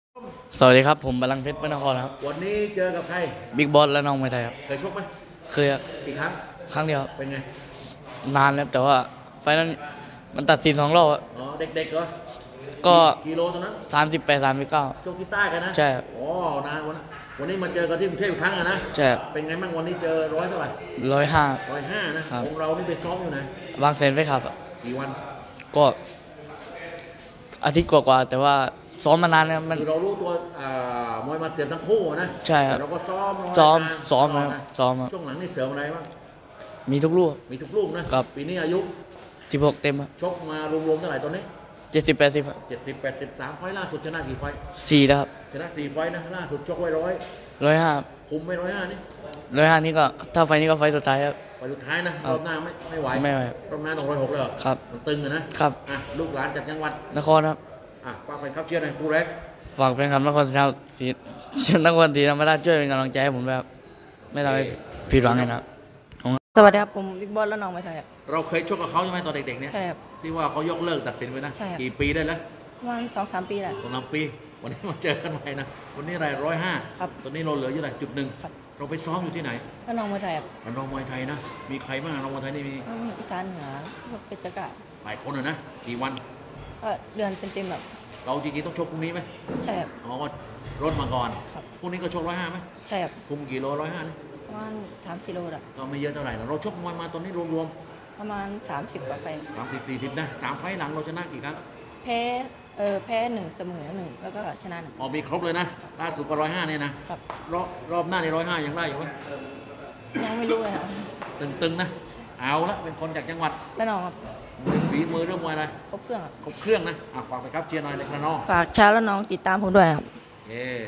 สัมภาษณ์ บัลลังค์เพชร vs บิ๊กบอส ศึกเพชรยินดี ช่อง true4u24 เริ่ม 18.00 น เวทีมวยราชดำเนิน
สัมภาษณ์ มวยไทยวันนี้ สัมภาษณ์ บัลลังค์เพชร vs บิ๊กบอส ศึกเพชรยินดี ช่อง true4u24 เริ่ม 18.00 น เวทีมวยราชดำเนิน (24-10-67)